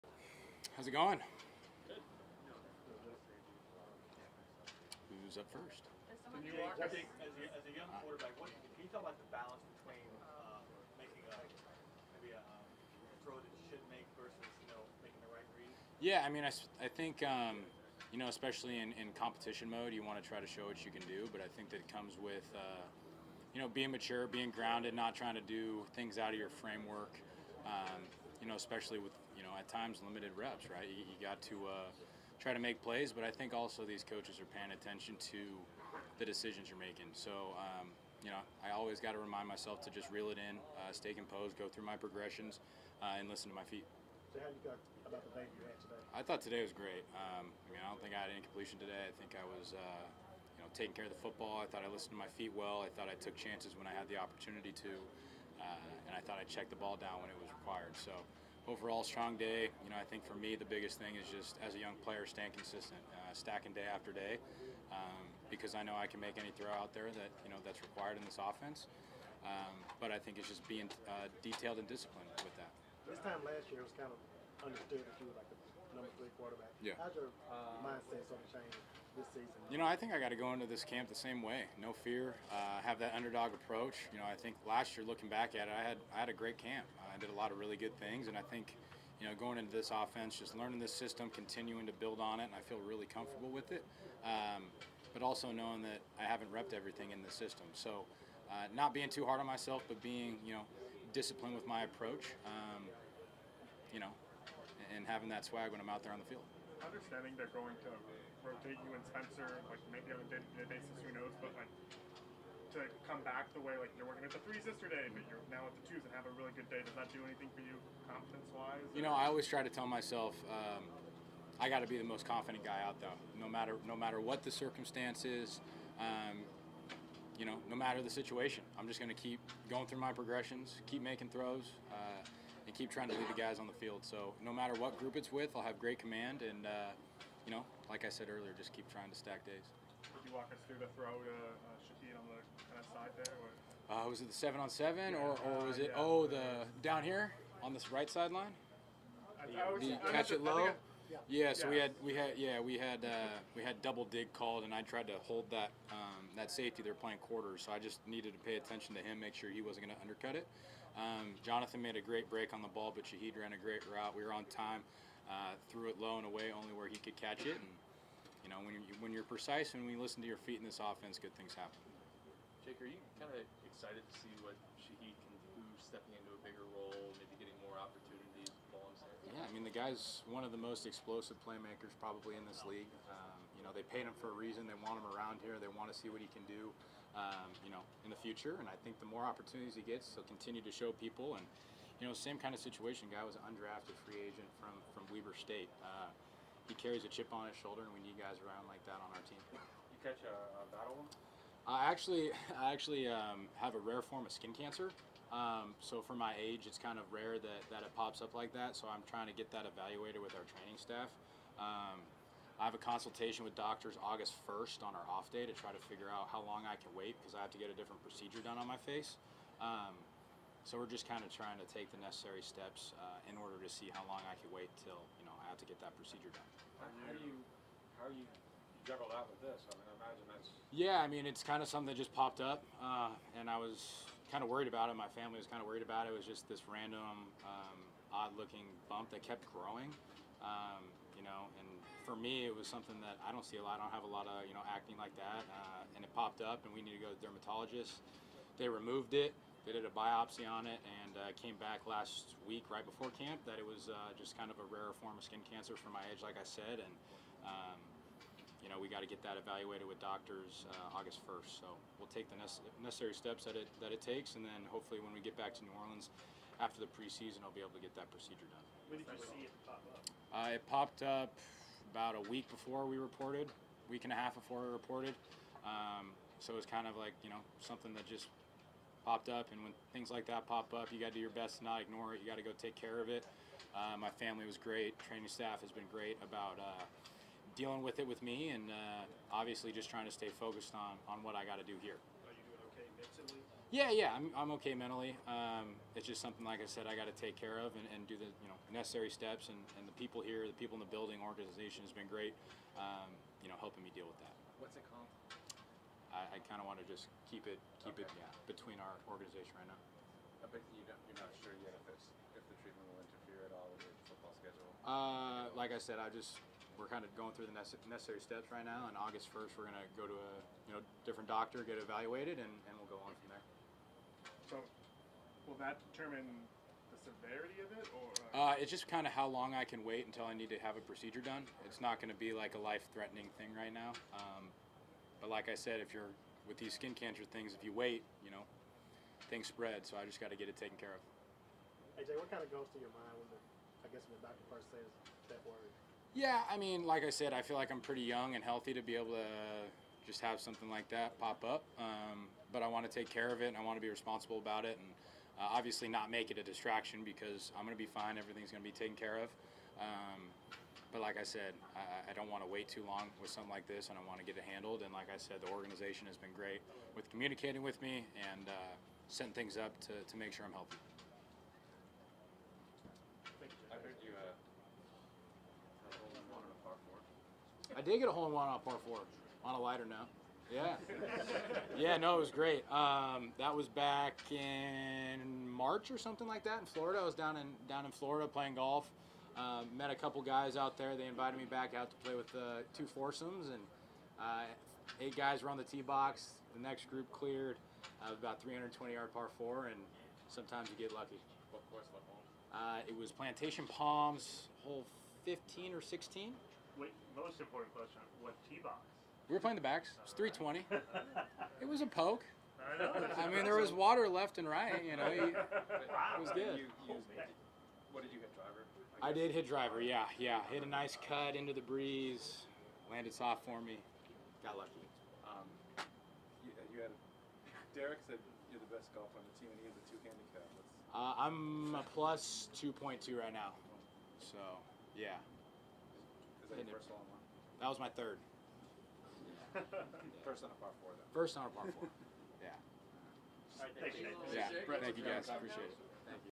Saints QB Jake Haener diagnosed with rare form of skin cancer
Haener was a fourth-round draft pick out of Fresno State in the 2023 NFL draft.